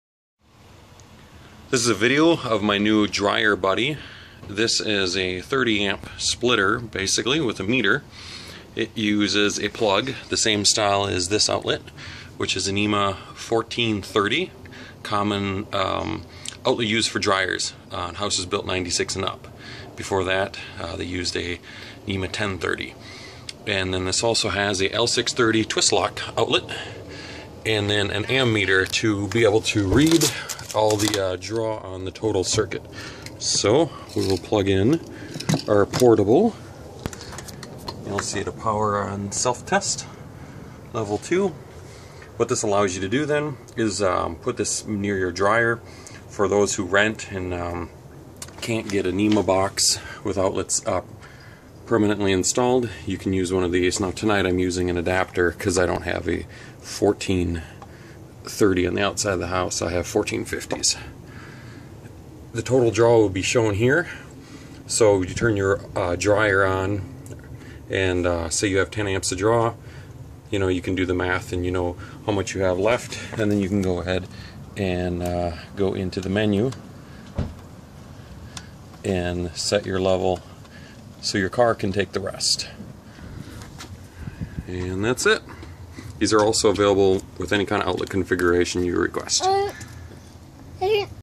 Custom Dryer Buddy #3 demo for Level 2 EV charging stations - demo by BSA Electronics
Electric Vehicle Charging Station accessory. EVSE NEMA 14-30 to NEMA 14-30 and L6-30 outlets with Volt/Amp meter. Allows sharing of 1996+ dryer outlet.